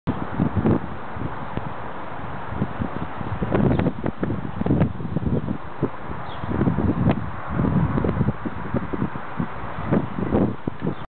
Copa nursing.MOV
Copa Cabana nursing. 4-24-10. Ft Collins, CO